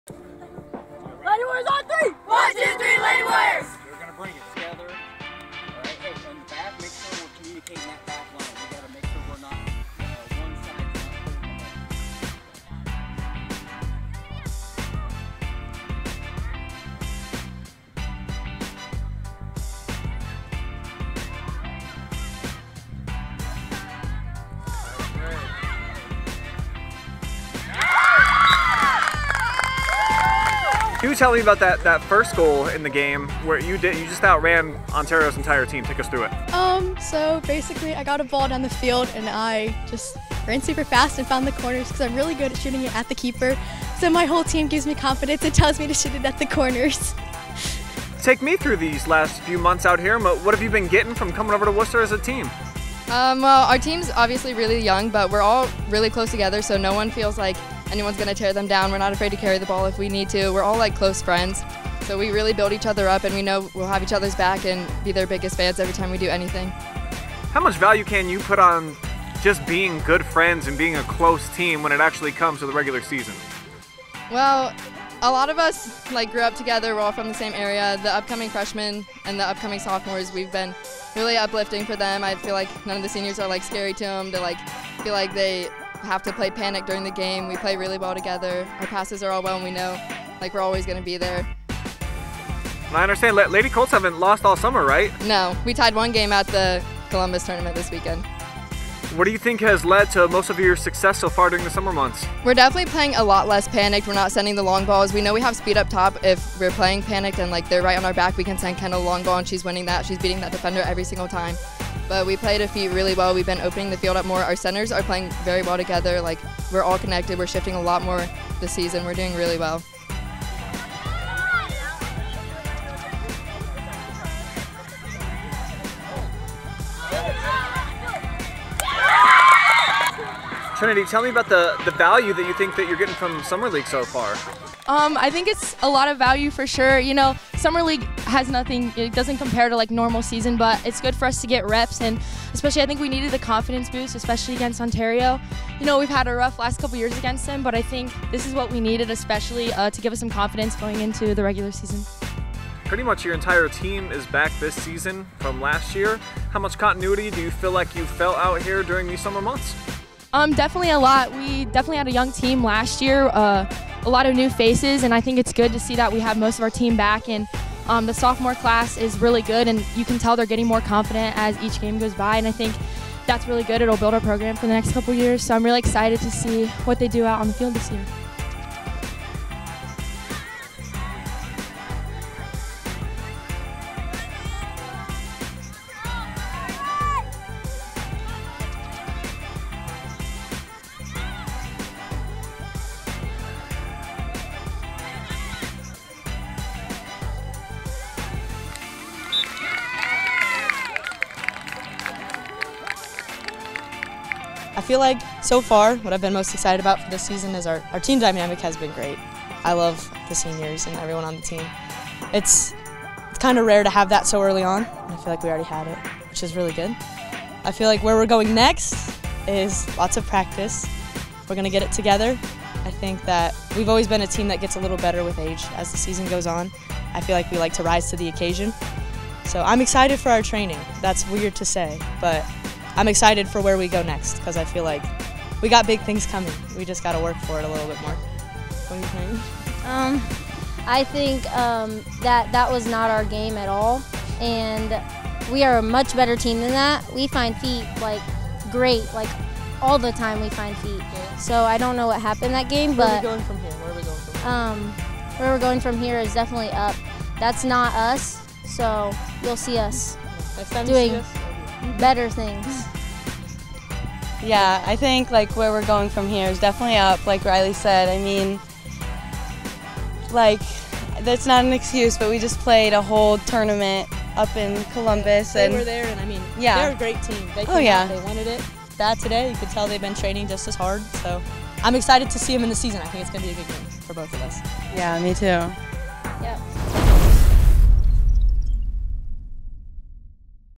MOAC Girls Soccer Contenders Meet In Summer Tune-up
Wooster has been the home for high school girls summer soccer on Mondays over the past month as area squads took the fields around the high school against other schools in the region.

For MOAC title contenders Clear Fork and Ontario, who met in a Division 2 District Semifinal last season, this summer is a time for both squads to prepare for a season that could see the two going head-to-head for a chance at a long tournament run and conference championship.

Watch highlights of the matchup as well as interviews with key pieces from both squads after the Lady Colts got the best of the Lady Warriors 3-1 at the summer league finale.

Presented By: Frito-Lay